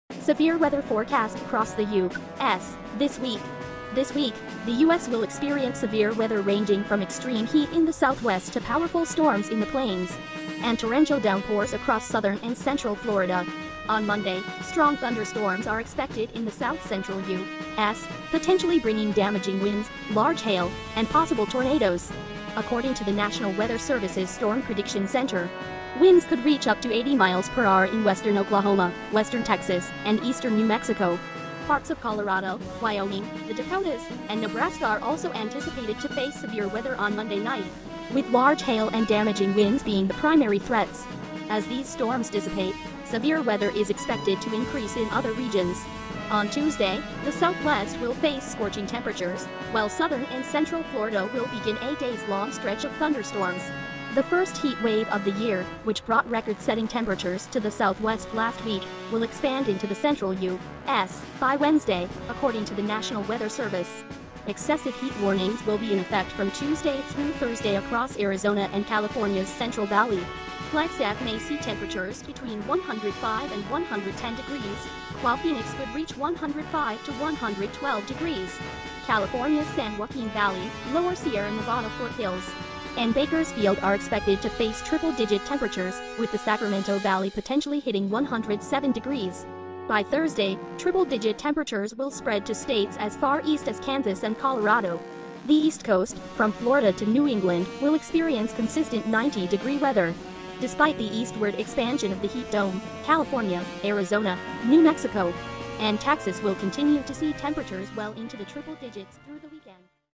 Severe Weather Alerts | What to Expect This Week Across the USA | Today USA Weather update
**Severe Weather Forecast Across the U.S. This Week**

This week, the U.S. will experience severe weather ranging from extreme heat in the Southwest to powerful storms in the Plains and torrential downpours across southern and central Florida.

On Monday, strong thunderstorms are expected in the south-central U.S., potentially bringing damaging winds, large hail, and possible tornadoes, according to the National Weather Service's Storm Prediction Center. Winds could reach up to 80 mph in western Oklahoma, western Texas, and eastern New Mexico. Parts of Colorado, Wyoming, the Dakotas, and Nebraska are also anticipated to face severe weather on Monday night, with large hail and damaging winds being the primary threats.

As these storms dissipate, severe weather is expected to increase in other regions. On Tuesday, the Southwest will face scorching temperatures, while southern and central Florida will begin a days-long stretch of thunderstorms.

The first heat wave of the year, which brought record-setting temperatures to the Southwest last week, will expand into the central U.S. by Wednesday, according to the National Weather Service. Excessive-heat warnings will be in effect from Tuesday through Thursday across Arizona and California's Central Valley. Flagstaff may see temperatures between 105 and 110 degrees, while Phoenix could reach 105 to 112 degrees. California's San Joaquin Valley, lower Sierra Nevada foothills, and Bakersfield are expected to face triple-digit temperatures, with the Sacramento Valley potentially hitting 107 degrees.

By Thursday, triple-digit temperatures will spread to states as far east as Kansas and Colorado. The East Coast, from Florida to New England, will experience consistent 90-degree weather. Despite the eastward expansion of the heat dome, California, Arizona, New Mexico, and Texas will continue to see temperatures well into the triple digits through the weekend.